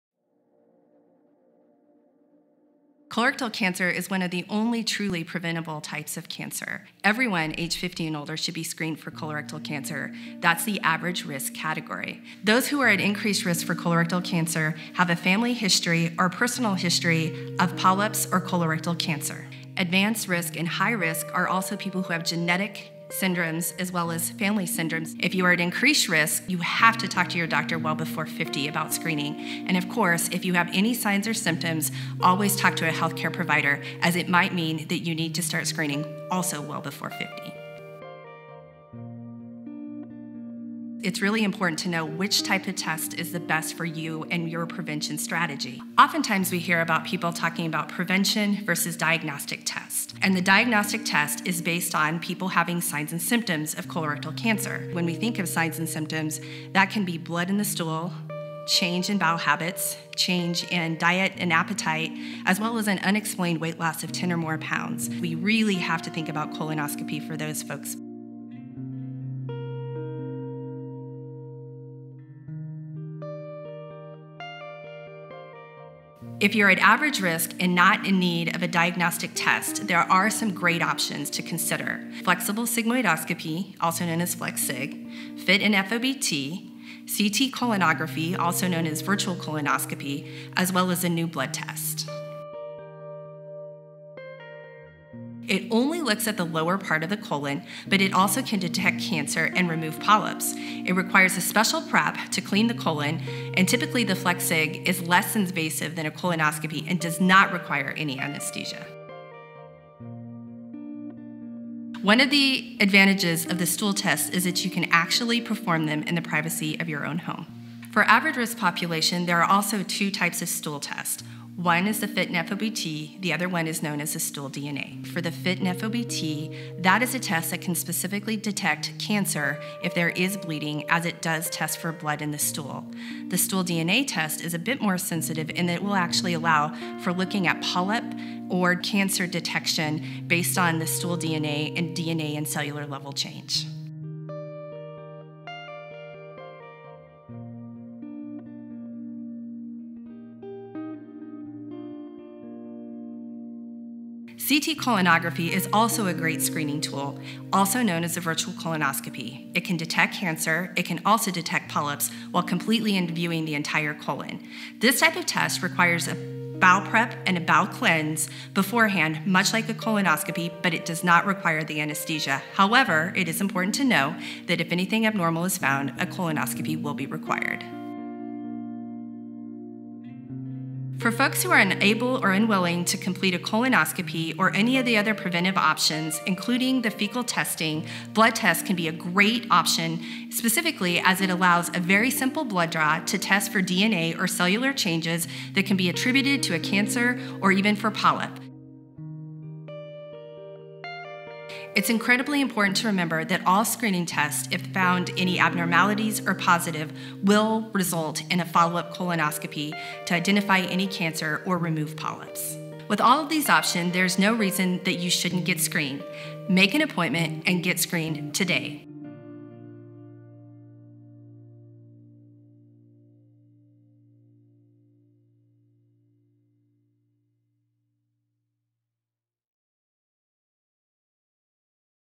Do I Need to Be Screened for Colorectal Cancer?
Fight CRC Director of Health Promotion Andrea (Andi) Dwyer covers who needs screened for colorectal cancer. She explains the difference between screening for prevention and diagnostic purposes. She also explains average risk and high risk. Andi covers the many options people have when it comes to colorectal cancer screening.